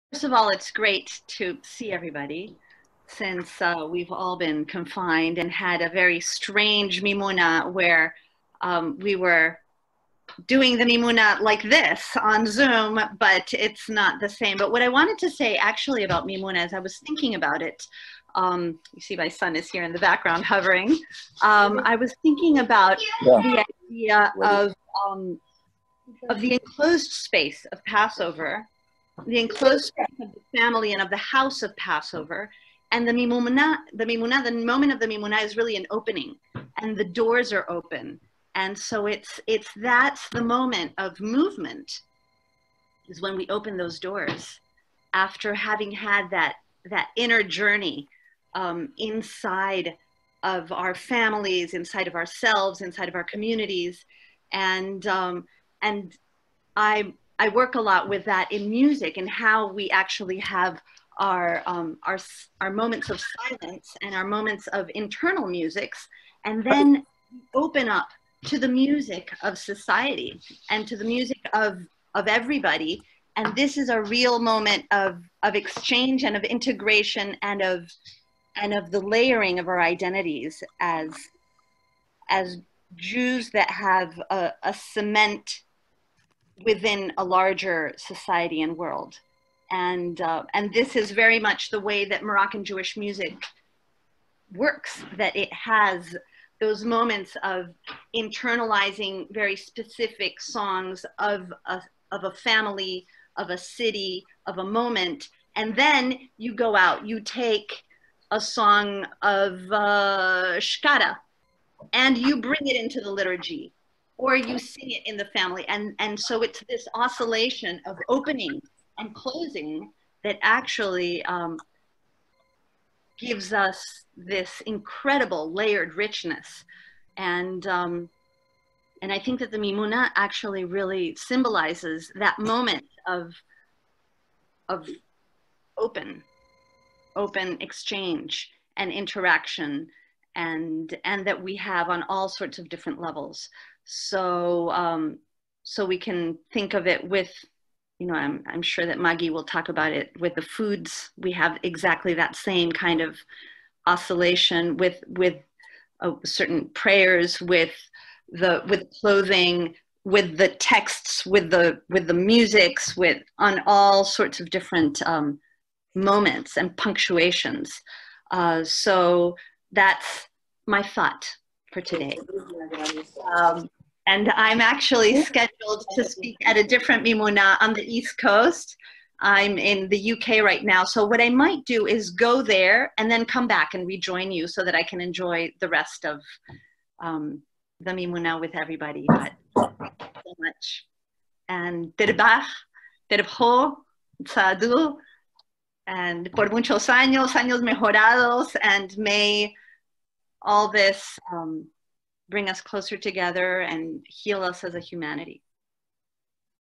Virtual Mimouna 2020: ASF Broome & Allen Fellow Dr. Vanessa Paloma Duncan-Elbaz from UK
Association Mimouna, the American Sephardi Federation, MALA (Muslim American Leadership Alliance), and MANY (Moroccan Americans in New York) organized a virtual Mimouna bringing together panelists from Morocco, Israel, the US, Canada, UK, and France.